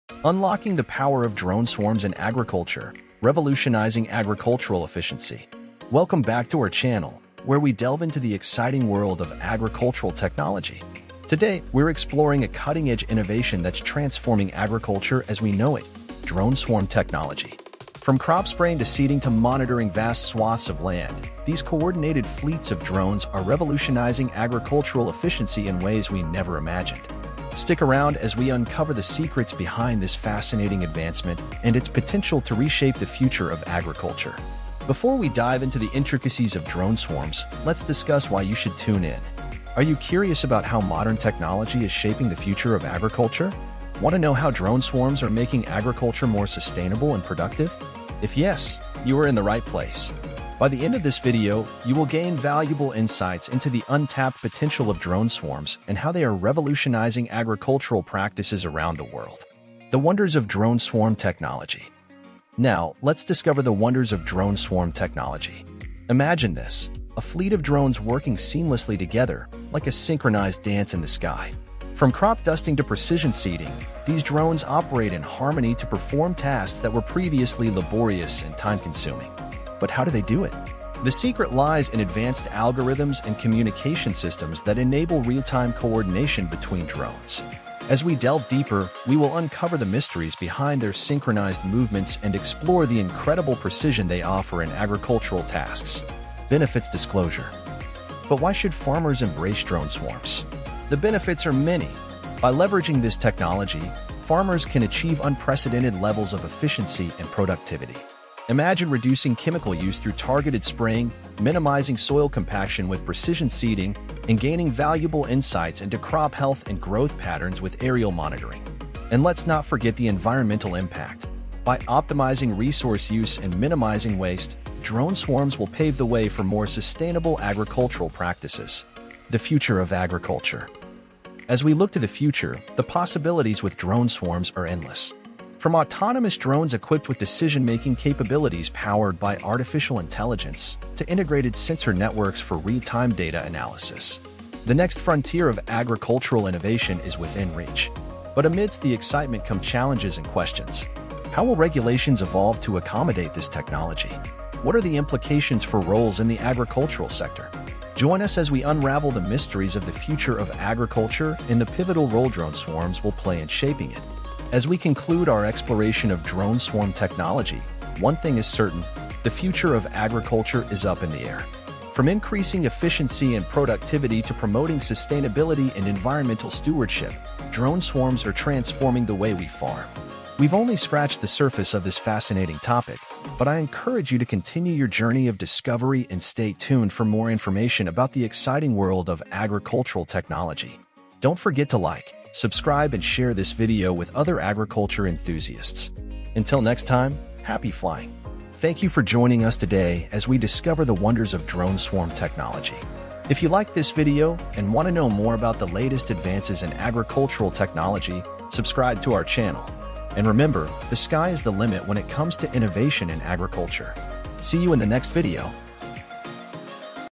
Unlocking the Power of Drone Swarms in Agriculture: Revolutionizing Agricultural Efficiency
Unlocking the Power of Drone Swarms in Agriculture: Revolutionizing Farming Efficiency

In this captivating video, we delve into the fascinating world of drone swarm technology and its profound impact on agriculture. 🌱

Join us as we uncover the secrets behind coordinated fleets of drones revolutionizing farming practices. From precision crop spraying to efficient seeding and aerial monitoring, drone swarms are transforming the way we cultivate the land. 🚜✨

Discover the marvels of drone swarm technology and how advanced algorithms enable seamless coordination among multiple drones. Learn about the benefits for farmers, including increased efficiency, productivity, and sustainability. 🌾💡

As we peer into the future, explore the limitless possibilities with drone swarms and the pivotal role they'll play in shaping the future of farming. From autonomous drones to integrated sensor networks, the sky's the limit for agricultural innovation! 🌍🚀

Don't miss out on this eye-opening exploration of drone swarm technology. Together, let's unlock the potential of drone swarms and revolutionize agriculture for generations to come! 🌟🌿